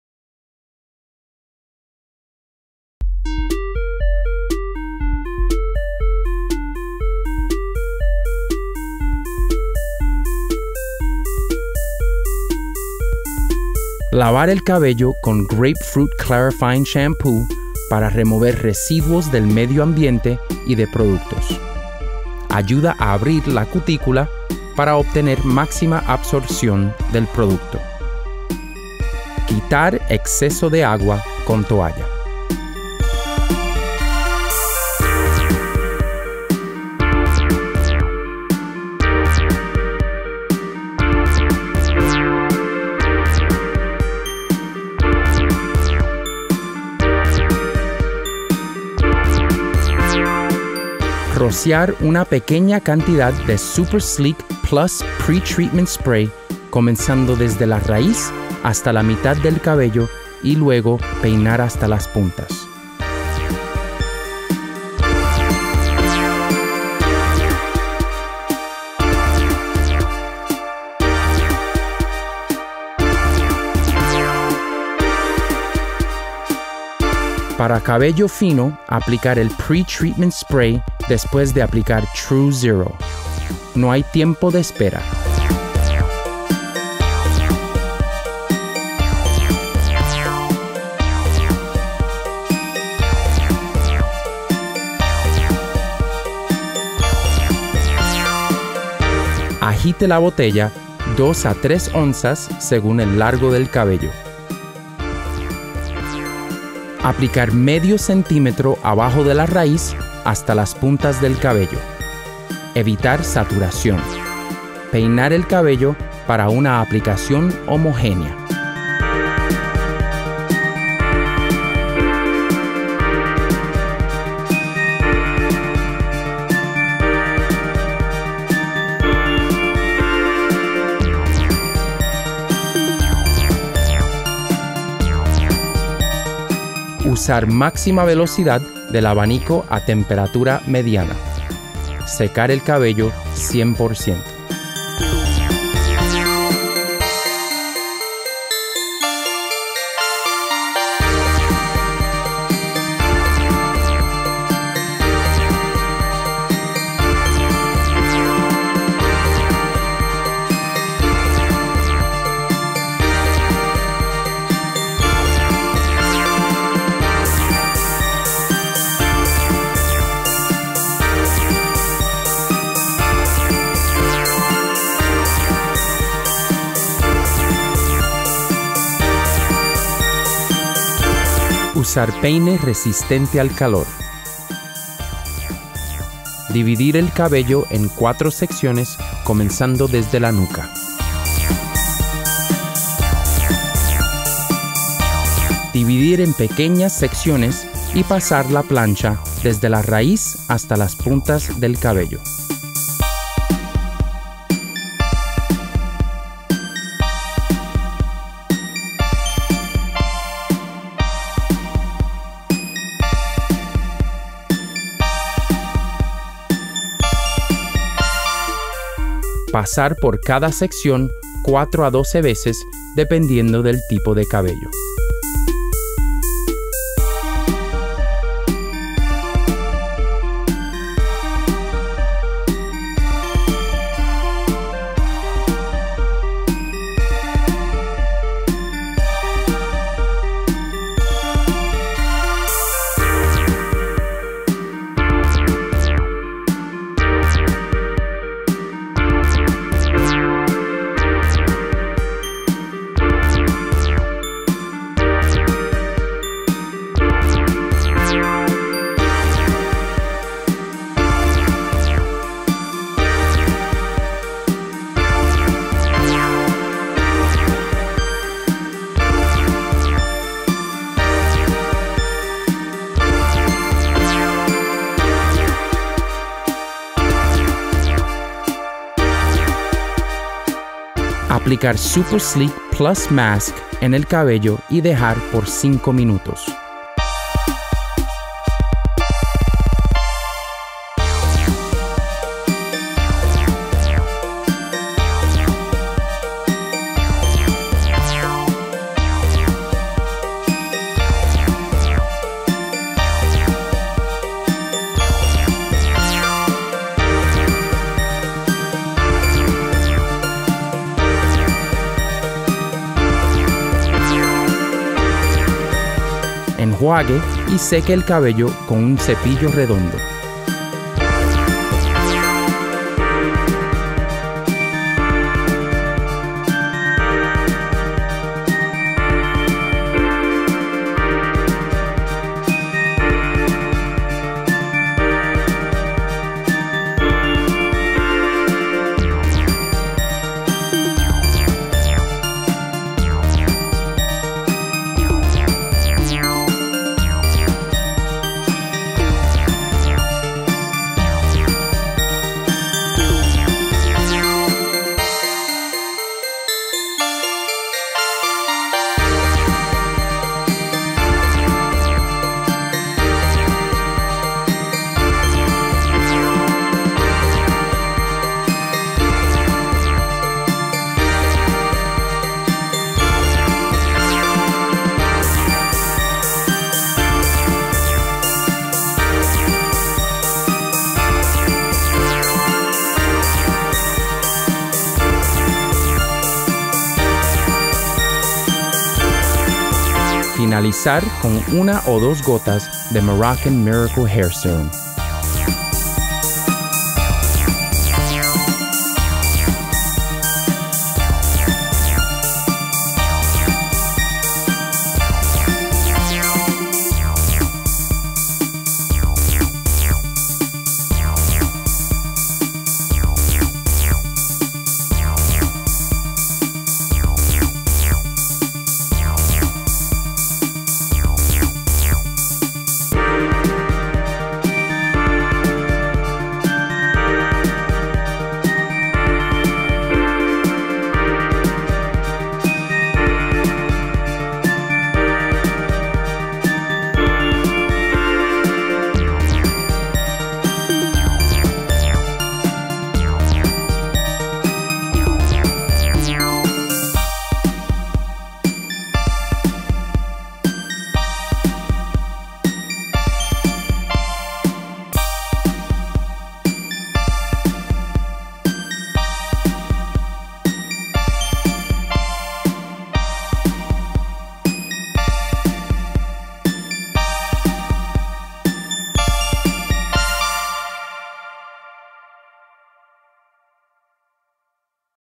True Keratin Eco Lux Hair Care - Spanish 1080p
True Zero Smoothing System
:: Formaldehyde Free and Made with Certified Organic Ingredients
Super Sleek+ Pre-Treatment Spray
Super Sleek+ Masque
Moroccan Miracle hair serum
Tint brush and bowl
Clips
Vent Brush
Easy Comb/Heat resistant comb
Blow Dryer
TK ProSmooth black titanium flat iron 450˚ (230C)